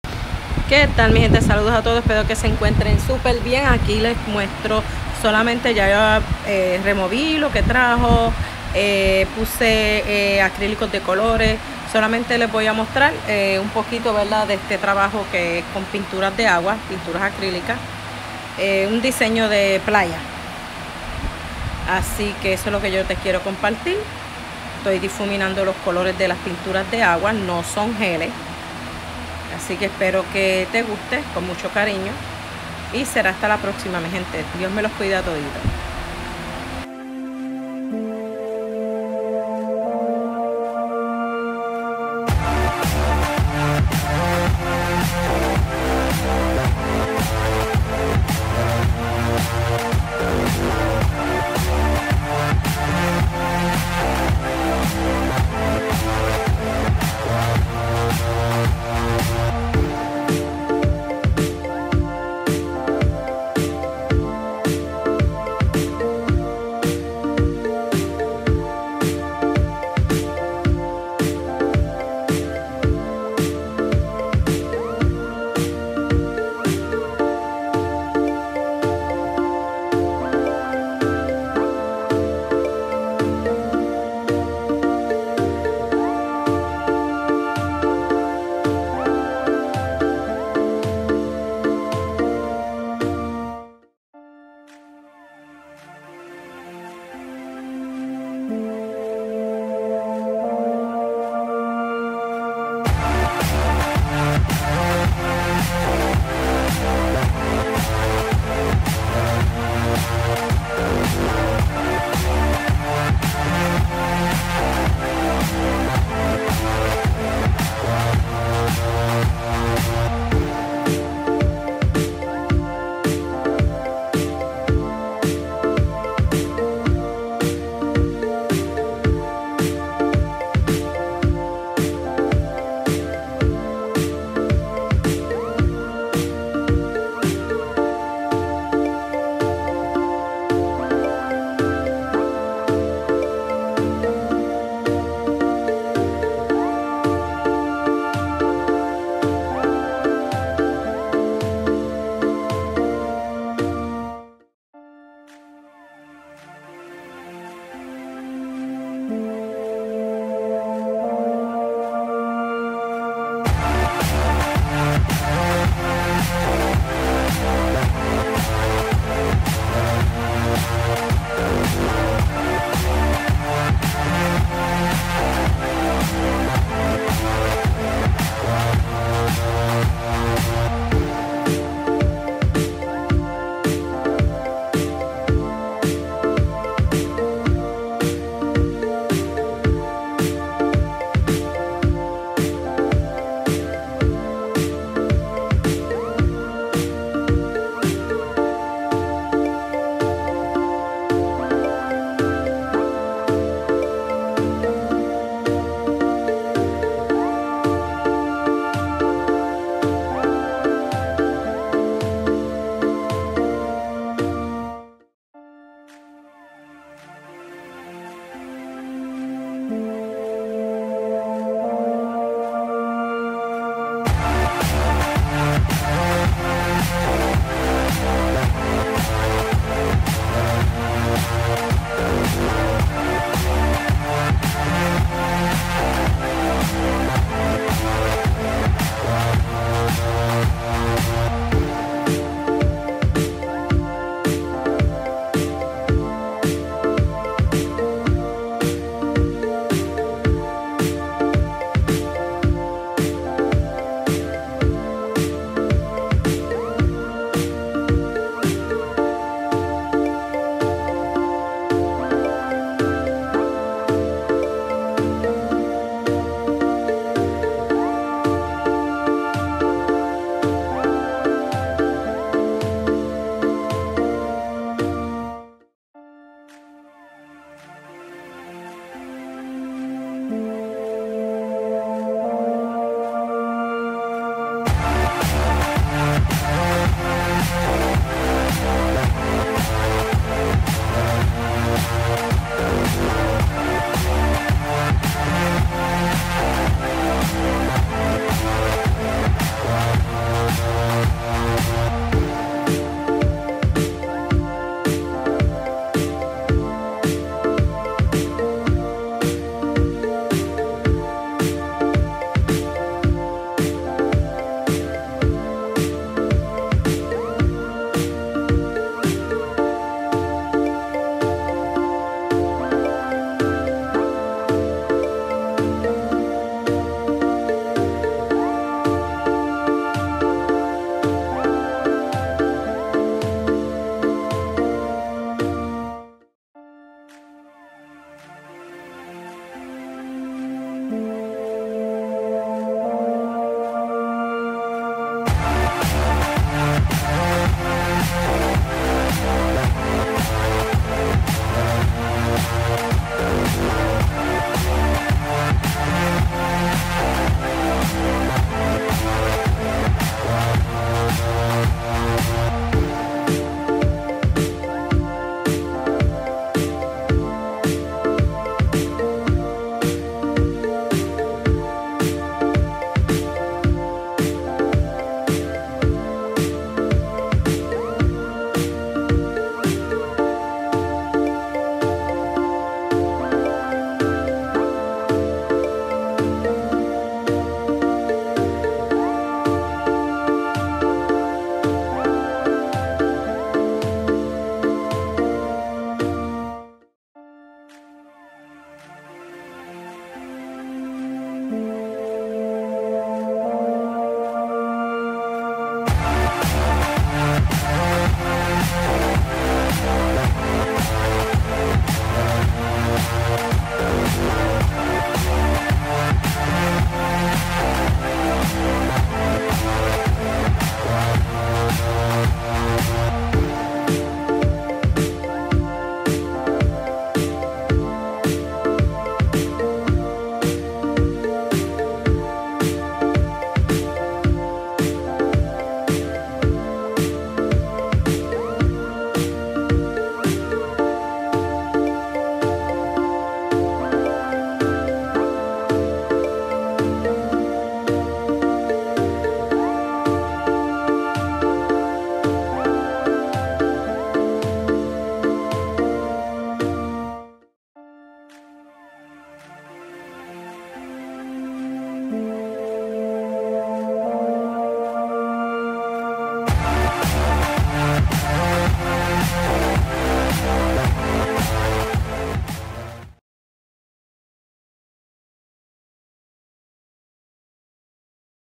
Mano alzada con pinturas de agua (pinturas acrílicas) y otros diseños mas
Saludos mi Gente!
Solo te muestro como realice esta pintura playera. muy sencilla. trate de limpiar bien los bordes. y ni con acetona me salieron...pero como quiera se los muestro, espero les guste.